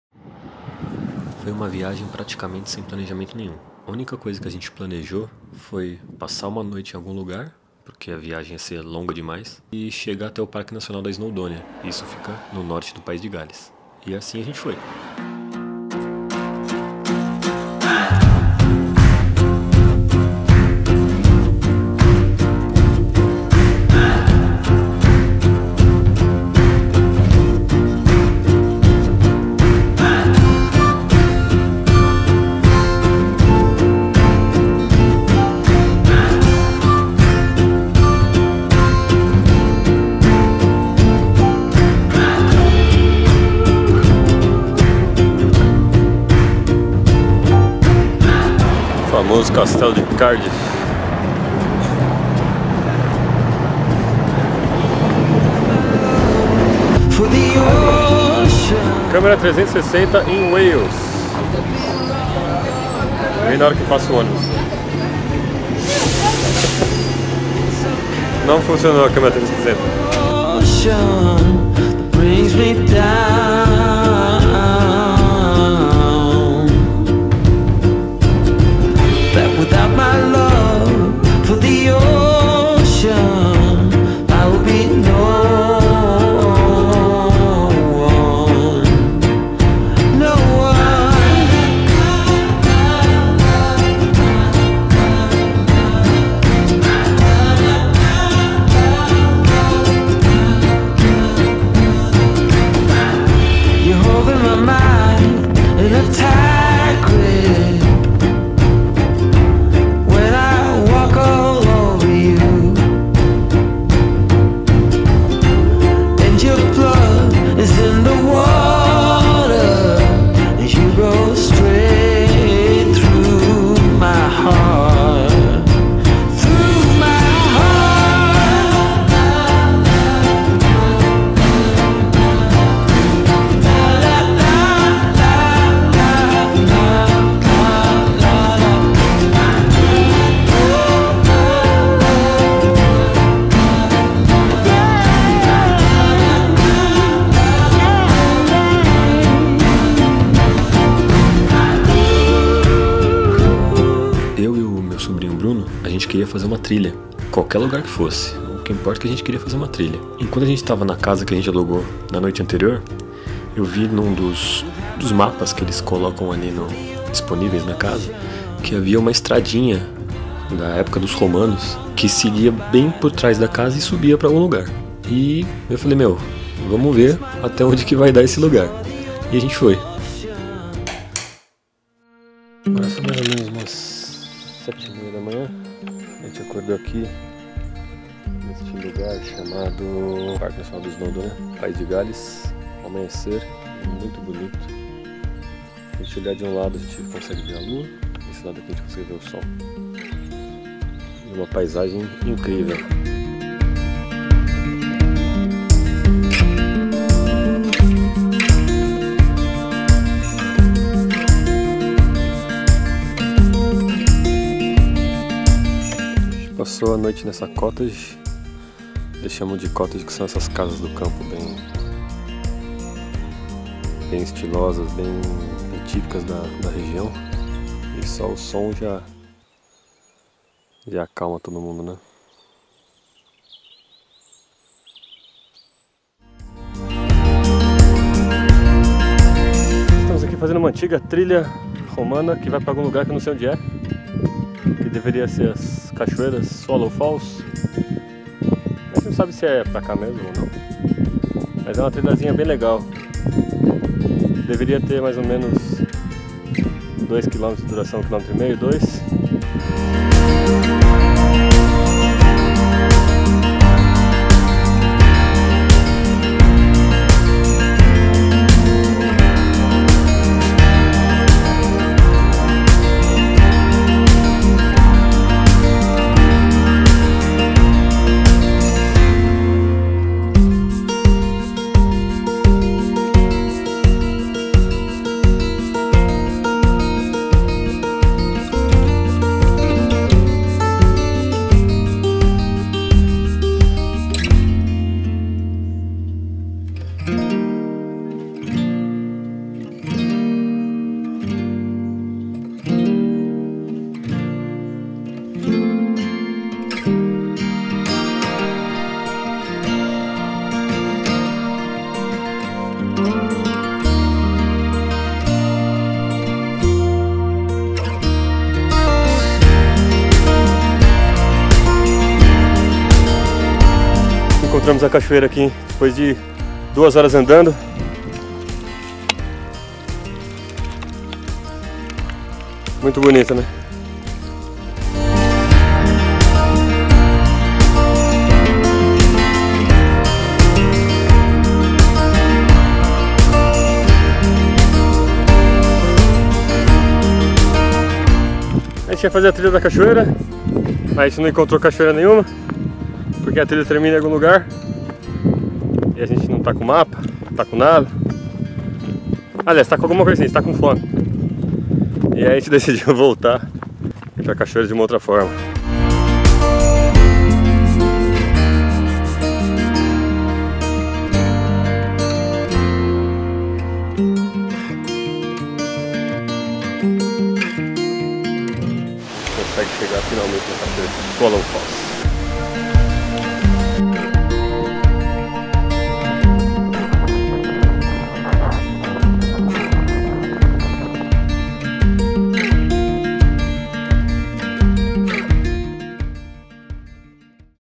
Você sabe o que tem no País de Gales? Parte 1
Nesse video eu mostro a viagem que fiz atravessando o País de Gales (Wales) até o Parque nacional de Snowdonia, uma pequena aventura de 2 dias passando por lugares fantásticos como a Cidade de Barmouth, o Castelo de Harlech, passar uma noite em uma Cottage House (Casa de campo tradicional), fazer uma antiga trilha romana, conhecer a Cachoeira de Swallow Falls e muito mais.

Meu Instagram : andreduartefilm
Minha Camera: Panasonic Lumix G7
Meu Drone: DJI Spark